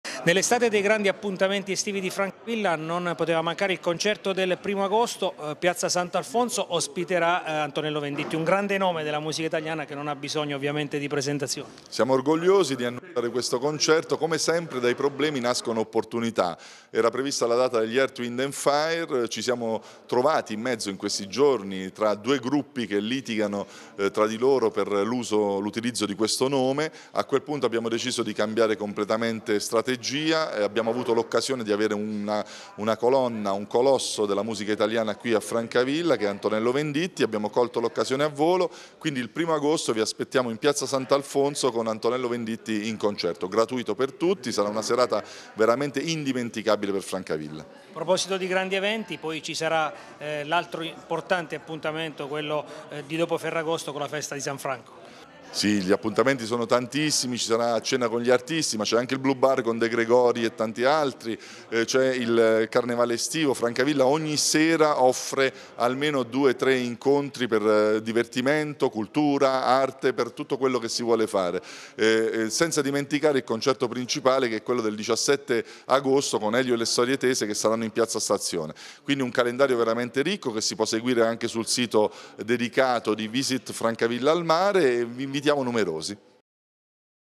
Colpo grosso, Venditti a Francavilla
Colpo grosso dell'Amministrazione Comunale di Francavilla al Mare: il 1 agosto in Piazza S. Alfonso concerto gratuito di Antonello Venditti